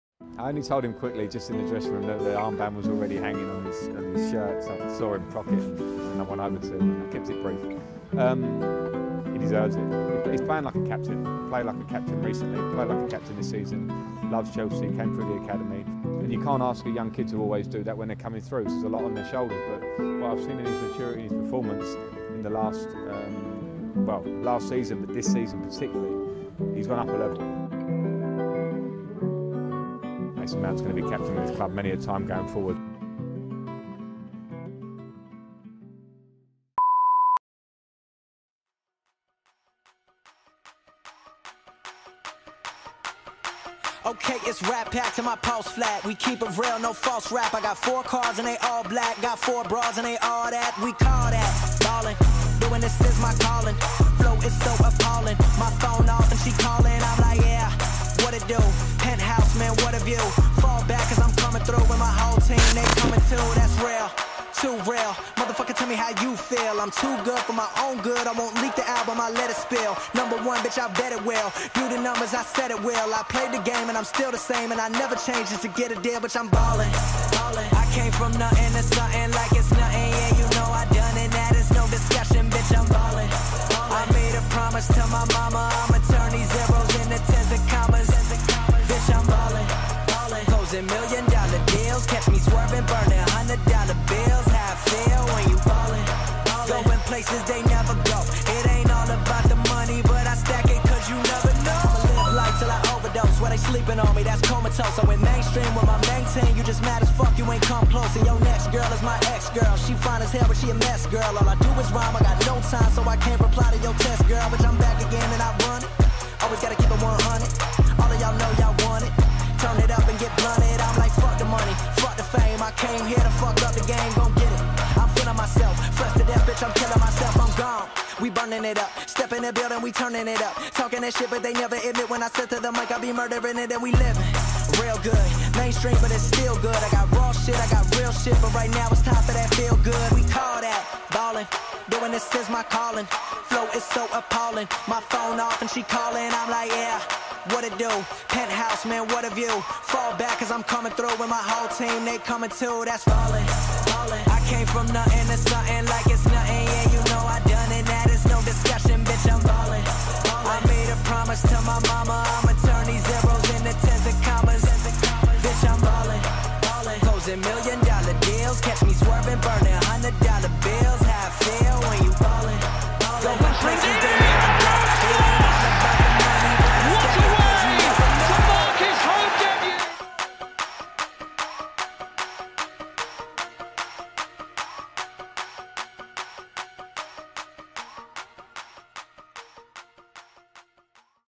Mason Mount, Cobham Made.
Not Lampard's, Southgate's or Tuchel's son, but a manager's dream. Presenting Cobham's latest jewel, Mason Tony Mount.
Had to re-upload due to some issues.